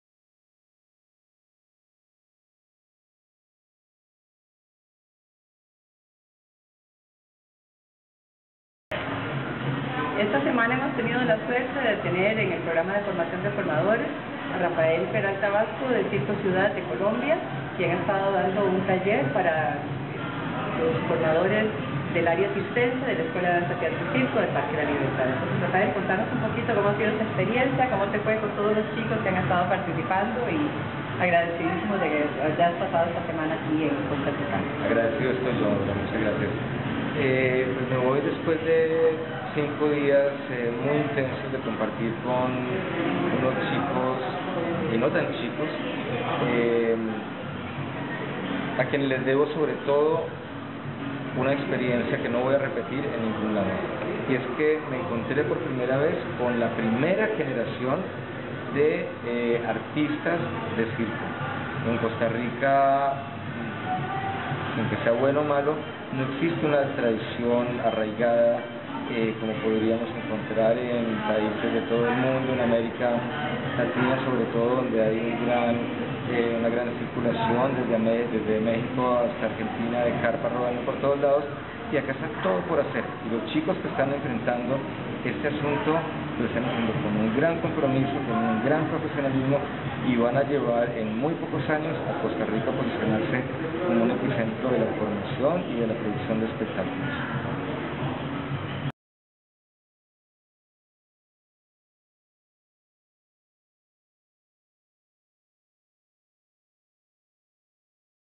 Encuentro de Profesionales de las Artes Cirsences: Rafael Peralta de Circo Ciudad
Rafael Peralta, Director General de la Fundación Escuela de Artes y Nuevo Circo - Circo Ciudad, de Bogotá Colombia, estuvo acompañándonos del 19 al 23 de septiembre de 2011 para enriquecer desde su experiencia la formación e implementación de la Escuela de Danza, Teatro y Circo del Parque La libertad